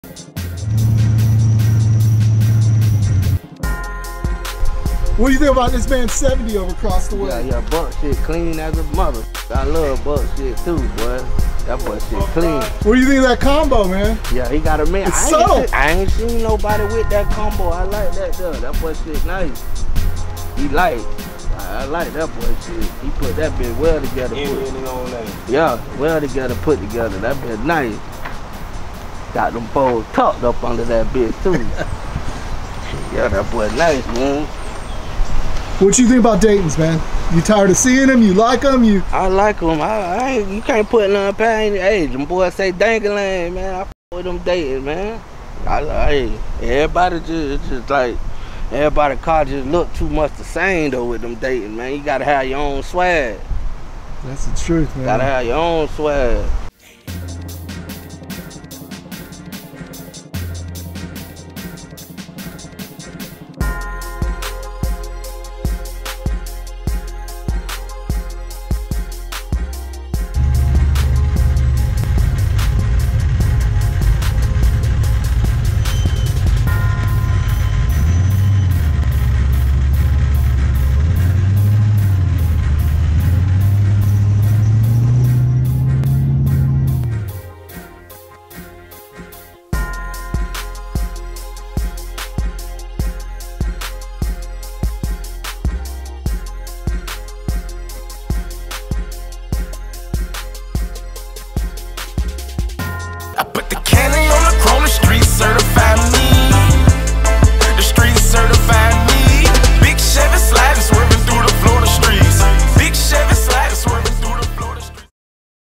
BUCK'S RARE 1970 IMPALA VERT
Buck from Broward County did his thing on this 1970 Impala Vert.
Silver with Green on Chrome Daytons. He brought something Fresh and Different to the streets.

☎Call Lance at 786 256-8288

🔎RELEVANT HASHTAGS:

DonkCar #1970 Impala